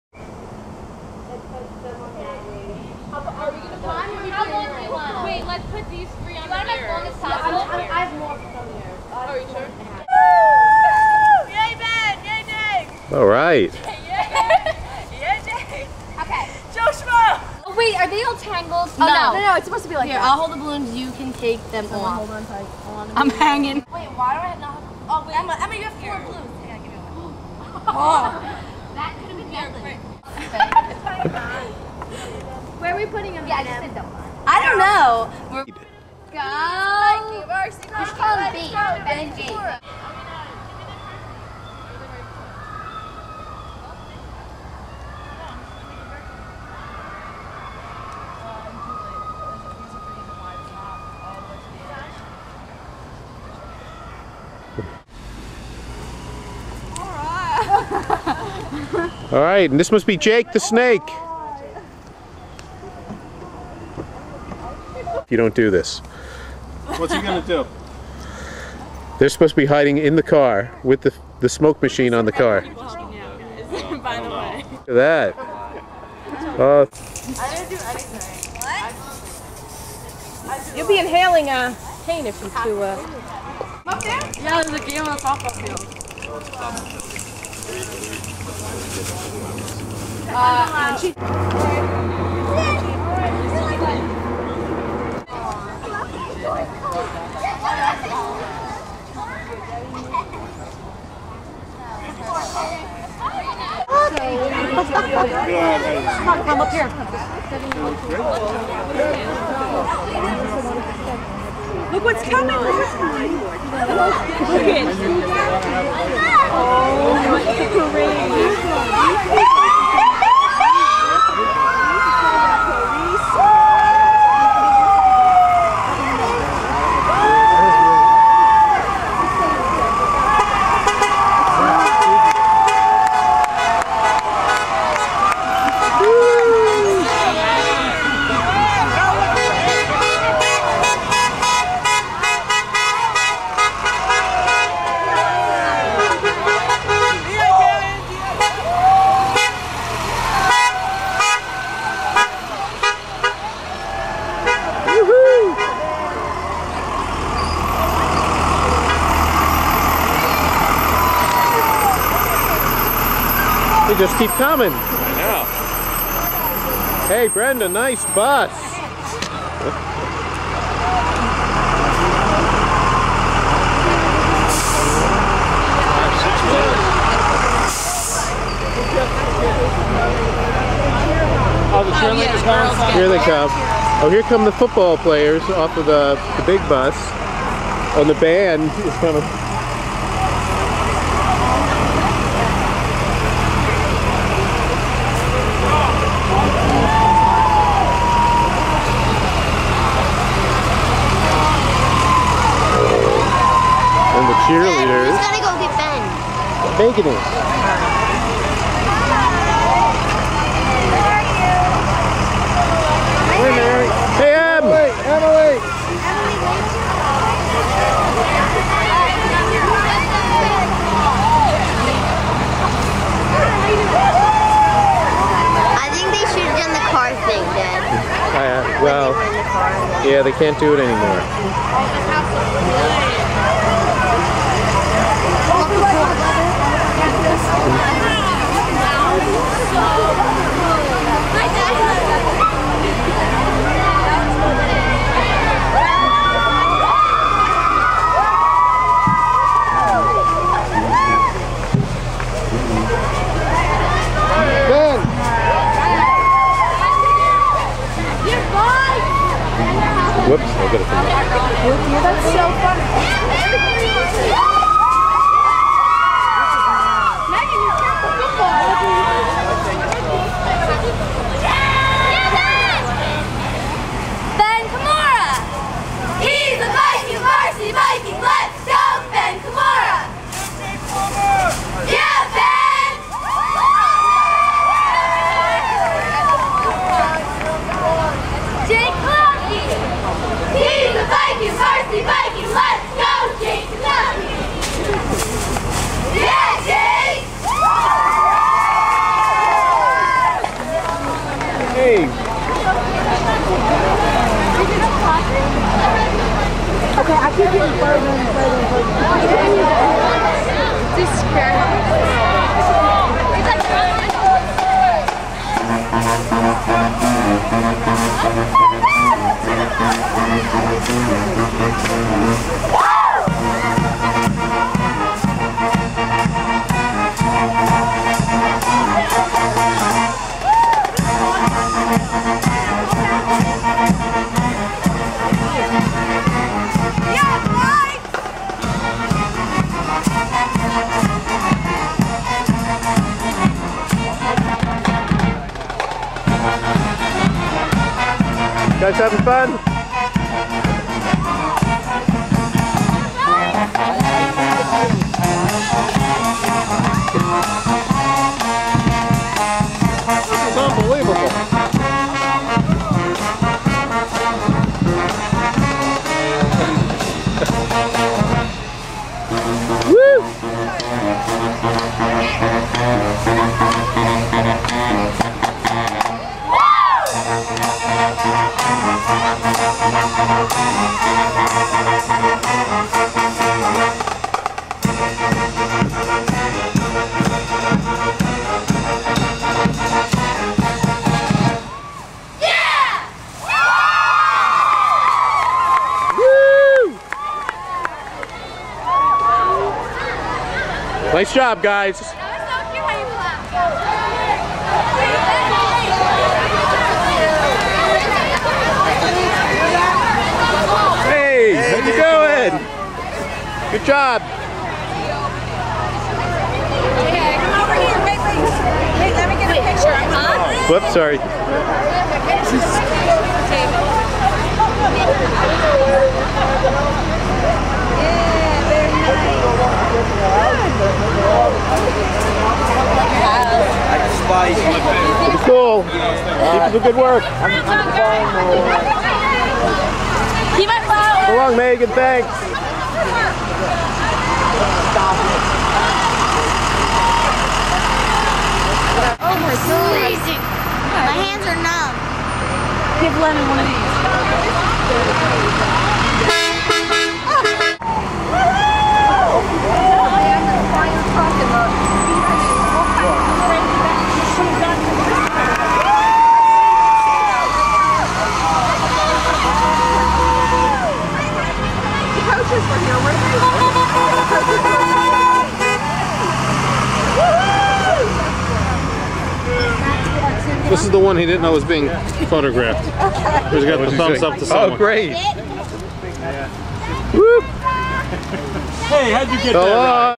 Vikings Motorcade 2008
Parsippany Hills Vikings Motorcade visits the Comora House on September 19, 2008